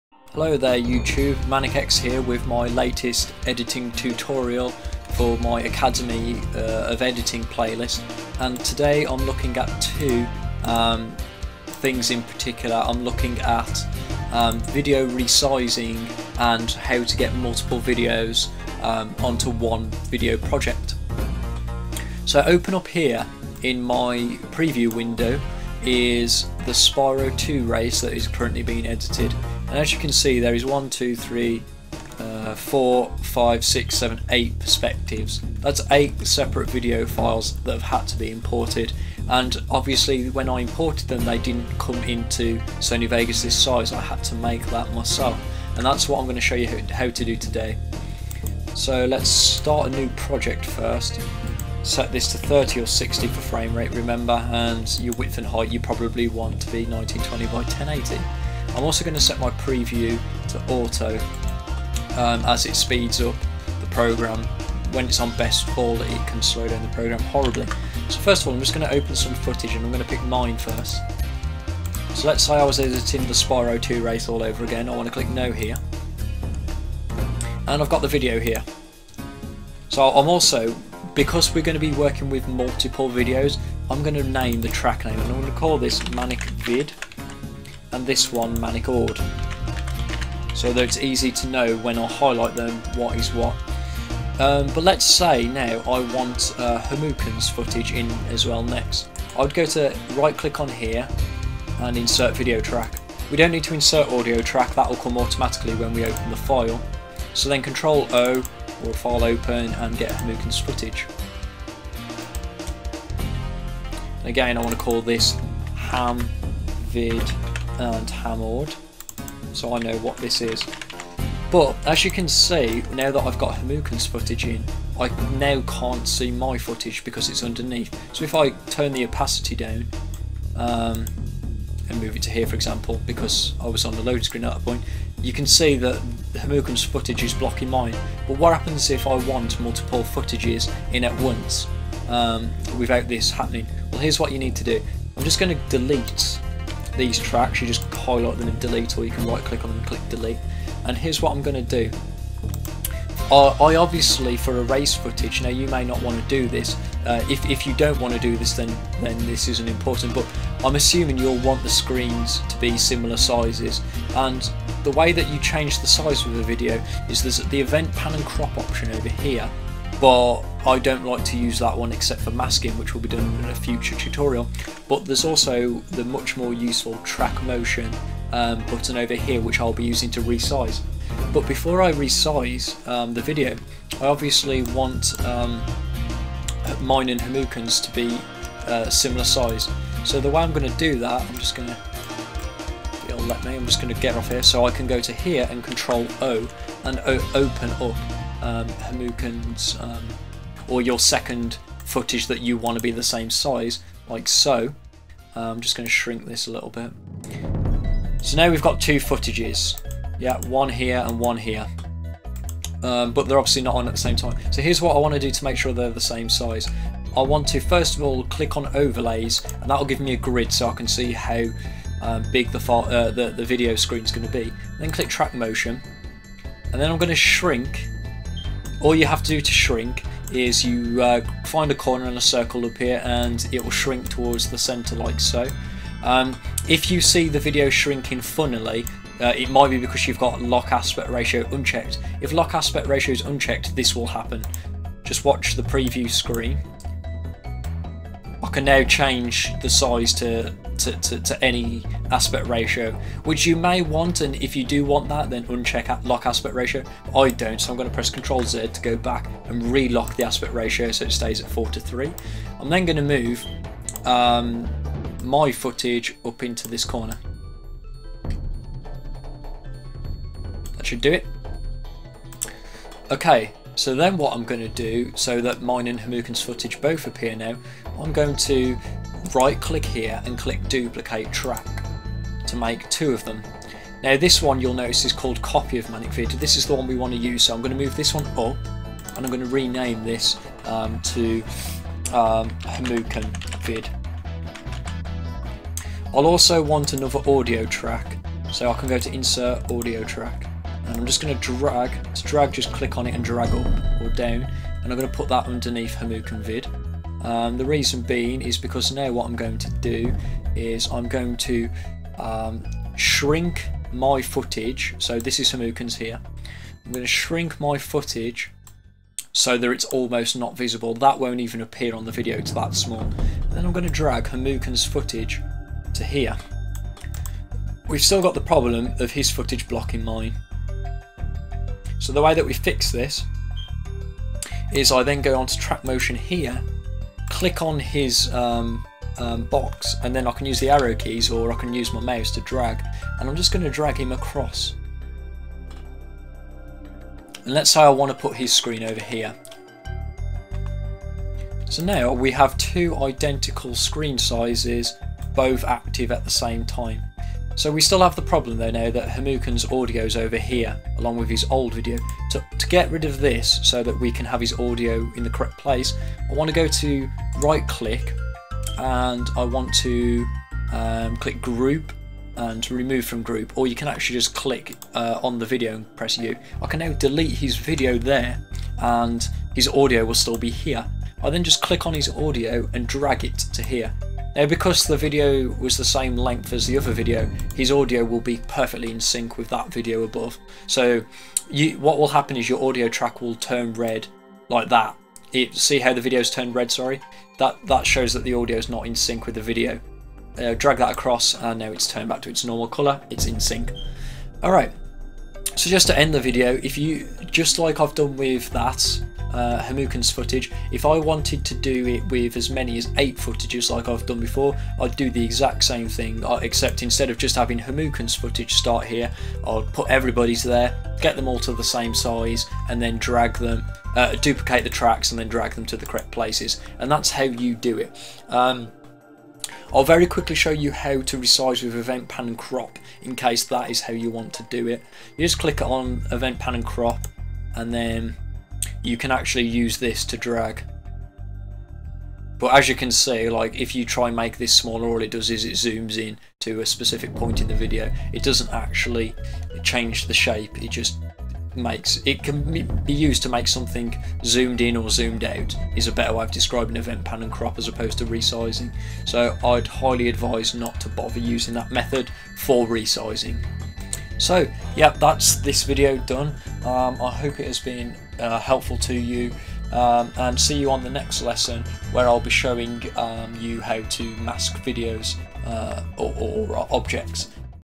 Sony Vegas - Lesson 3: Resizing Multiple Videos
Manic's Academy of Editing is back with Lesson Three.

Difficulty:  *  *

As a recap: The last two lessons consisted of looking at the basic interface of Sony Vegas, and then how we can ensure the quality we desire for our rendering.

This video looks at how to work with multiple videos, and how to resize videos. We will look at:

1. Inserting multiple video and audio tracks
2. Labelling tracks for easier navigation
3. Duplicating Tracks
4. Resizing videos using Track Motion
5. Locking and Unlocking Aspect Ratios
6. Utilising the Grid Overlay
7. Using Event Pan and Crop to alter the zooming focus of videos

Next Lesson: Masking